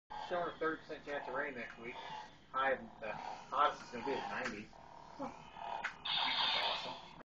Baby Jedi
Adalyn walking with lightsaber